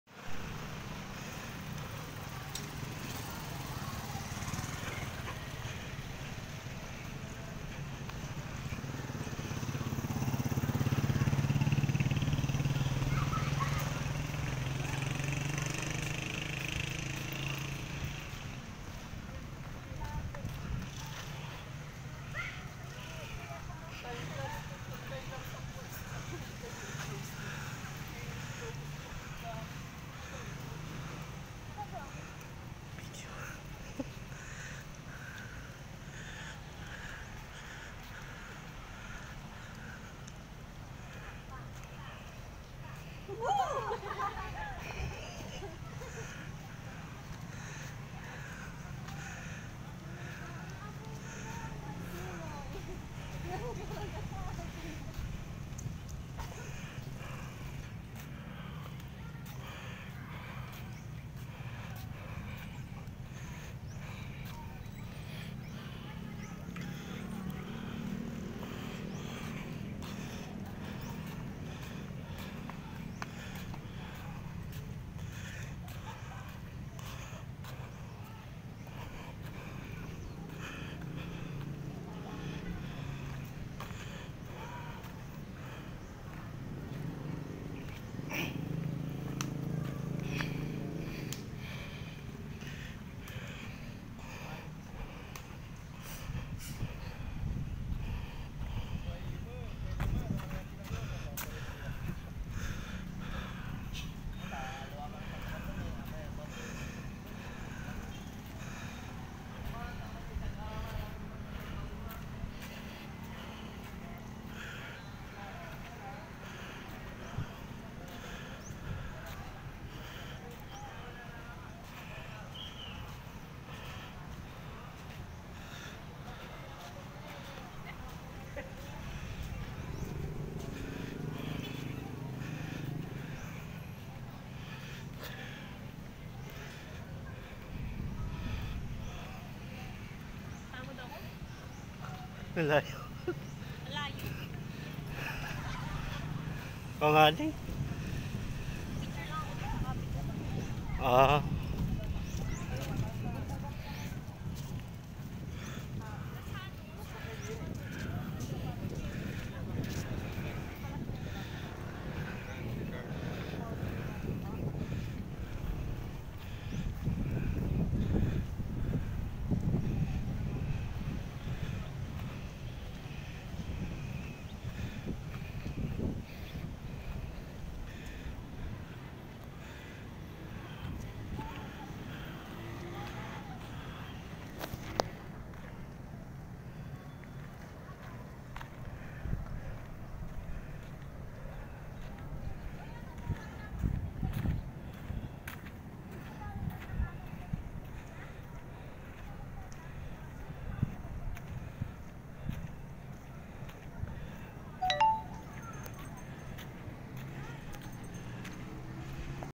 walkathon montage at the Capitol Lingayen, Pangasinan
random walking video around at the Capitol Lingayen, Pangasinan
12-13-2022